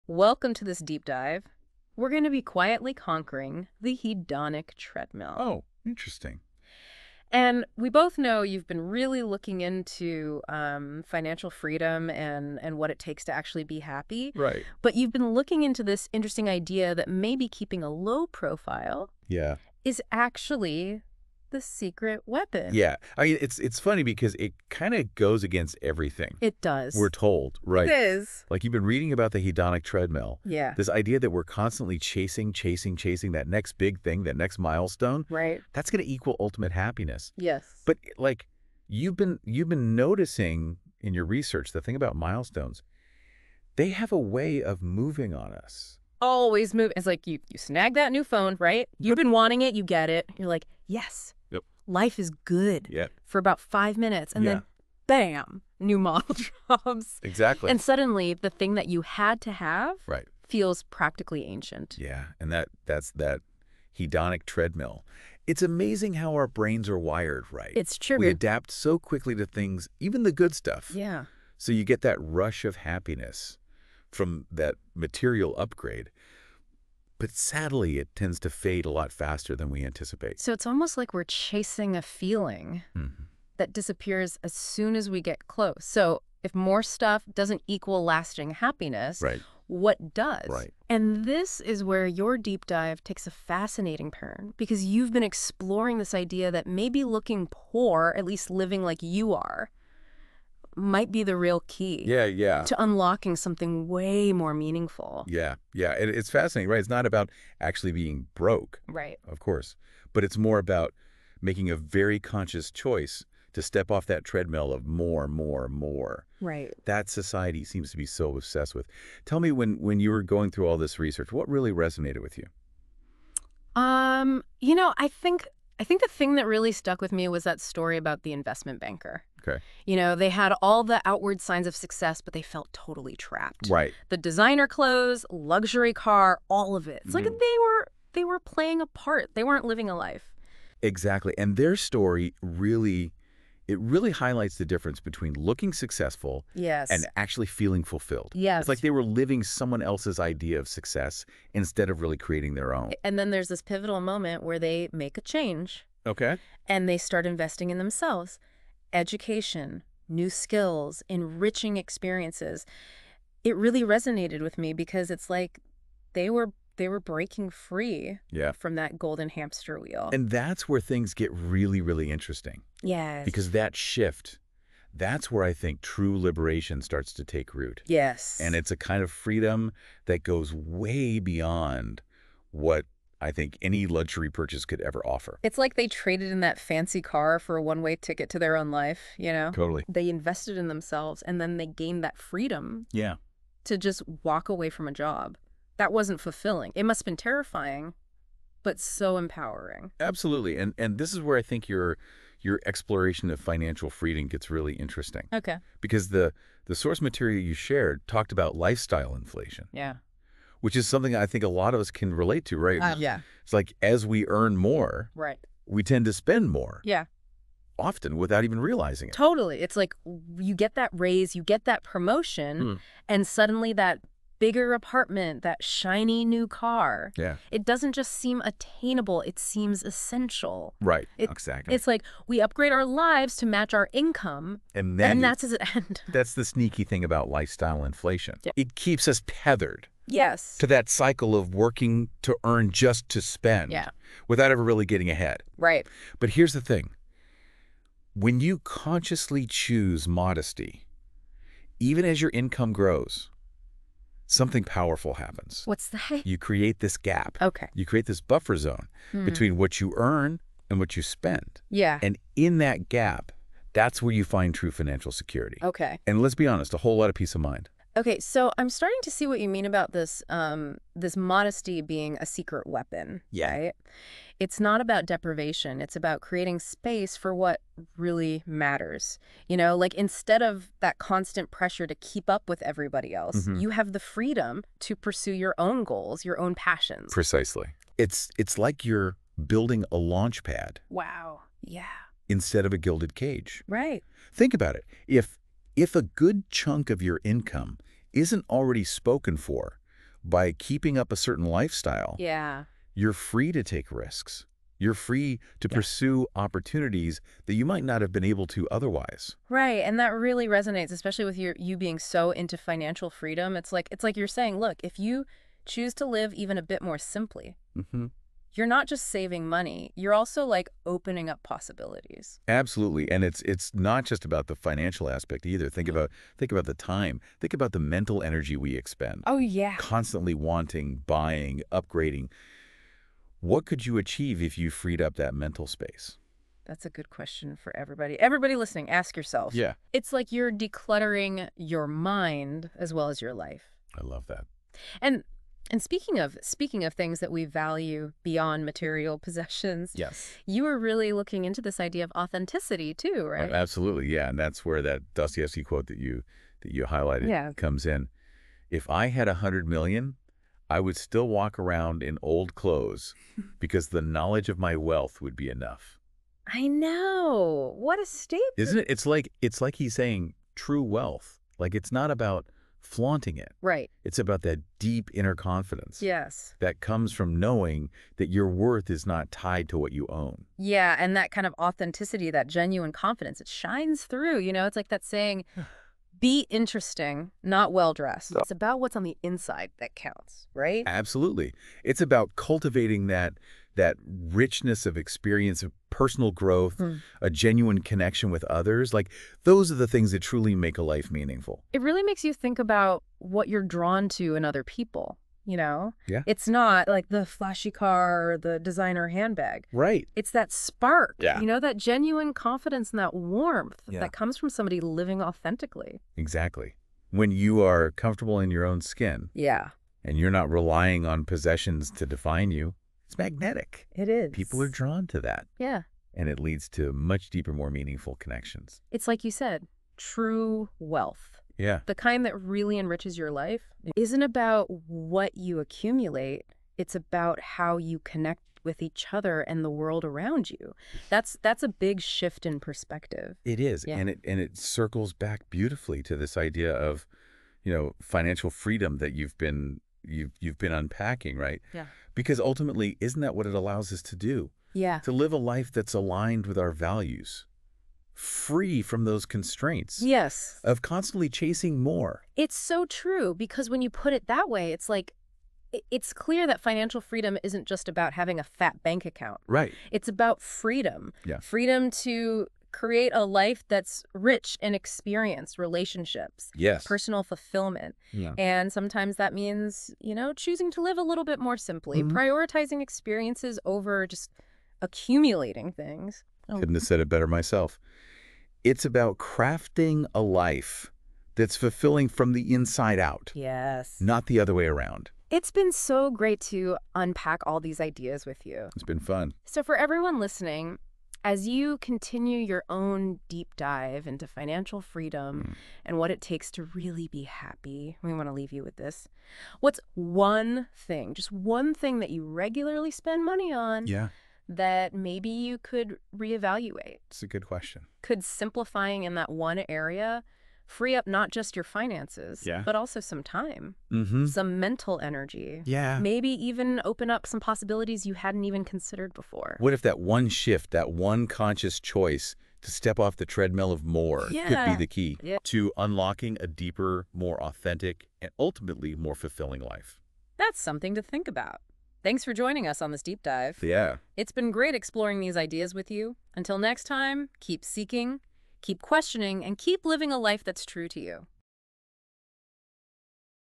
Why Appearing Poor Can Be the Smartest Financial Move
In this Podcast Video, we explore how keeping a low profile can unlock greater success and fulfillment. Stepping off the treadmill of constant comparisons frees you from dissatisfaction, amplifies financial freedom, and promotes authentic living. We break down the psychology behind materialism, the influence of social media, and how simplicity creates more happiness and flexibility. Gain practical strategies to break free from societal pressures and refocus on what truly matters for lasting rewards and peace of mind.

This podcast features AI-generated voices to present the content in an innovative way.